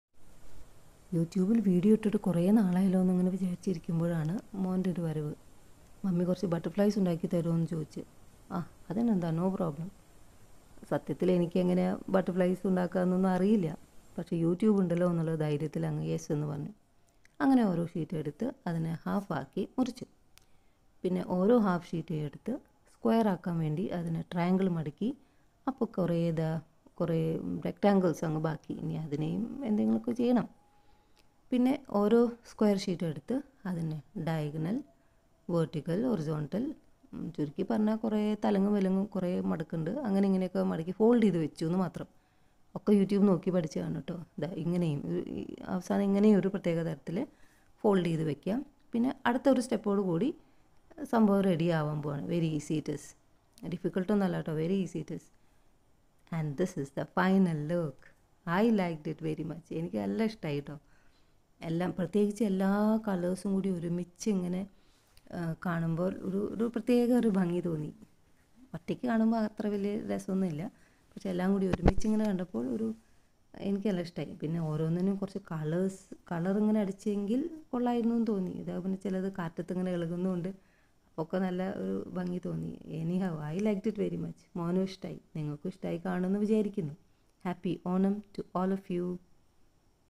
Paper butterflies
How I made some beautiful butterflies using A4 coloured scrape book sheets.
# butterflies making
origami butterflies